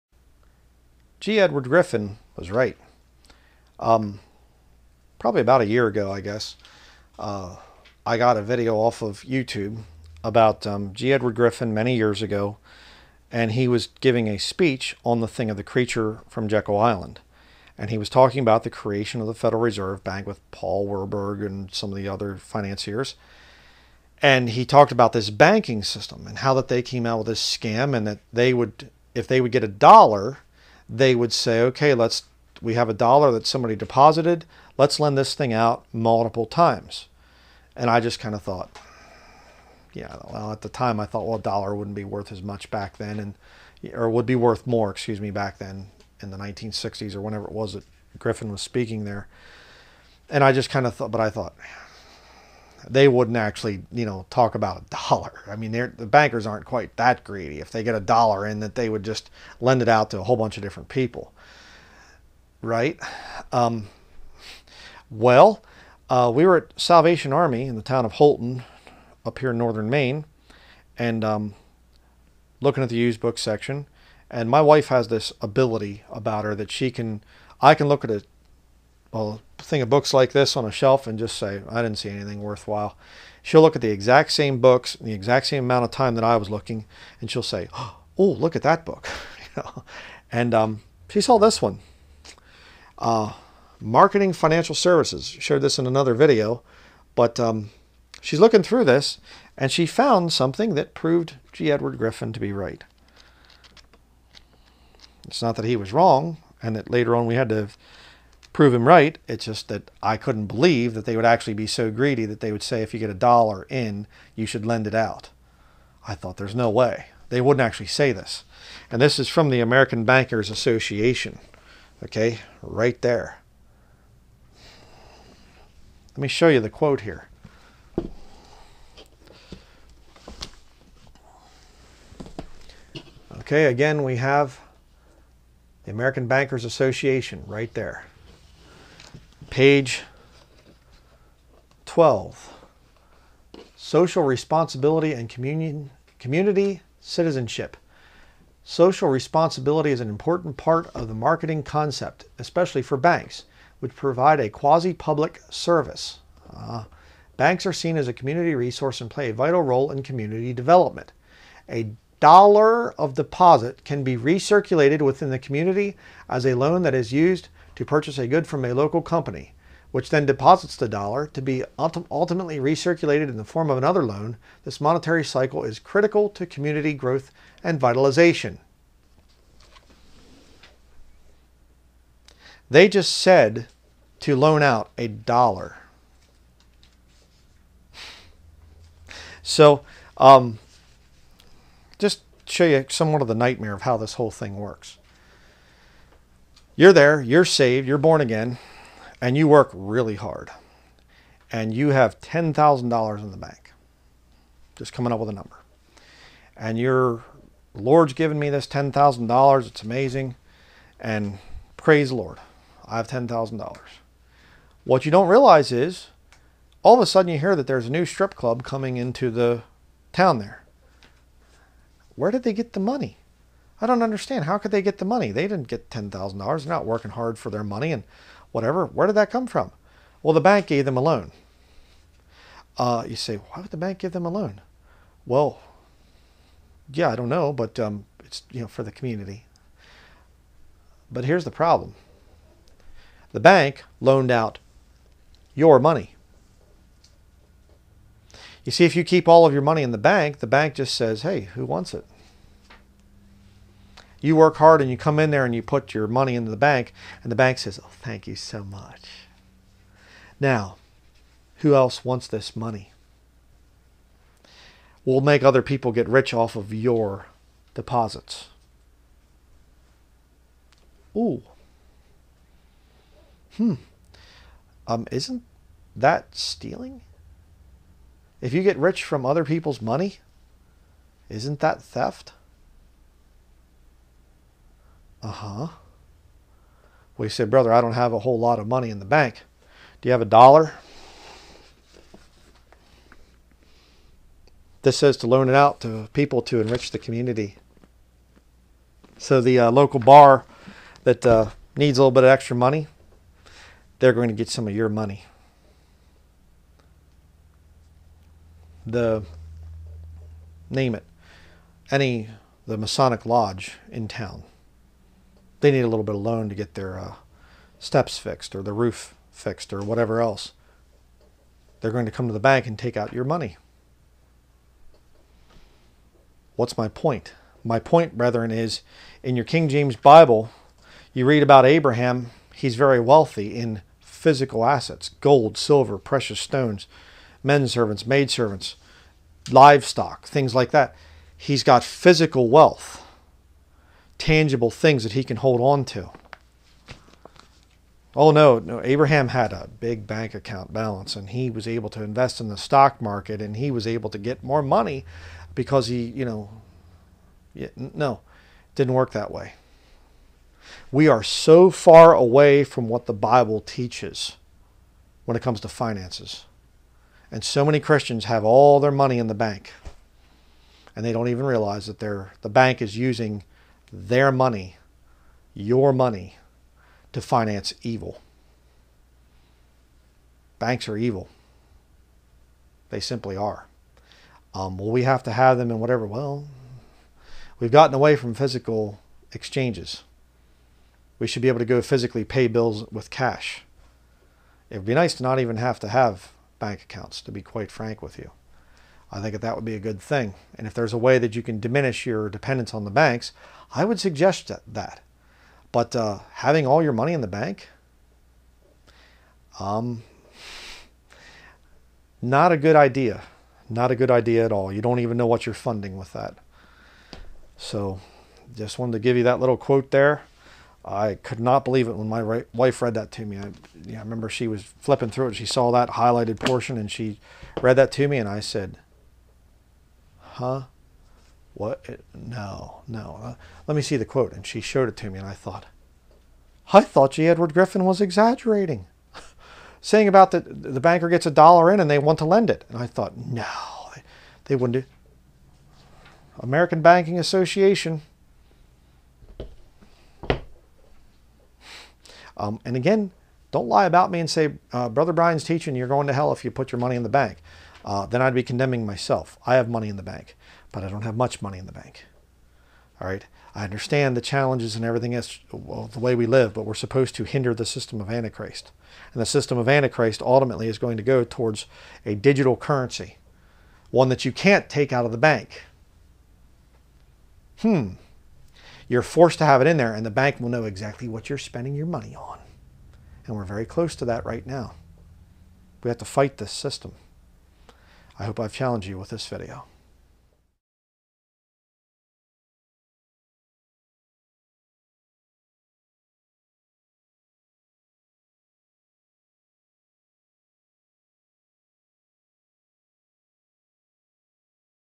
G. Edward Griffin Was Right About The Banking System!
We watched an old video of G. Edward Griffin as a family, in which he talked about the Federal Reserve and the corrupt banking system. In this speech he mentioned how banks will lend out deposits into their system, even when the deposit is as low as a dollar. I thought he was exaggerating to prove his point, until my wife found a copy of a manual from the American Bankers Association. This book comes right out and confirms what Griffin said many years ago!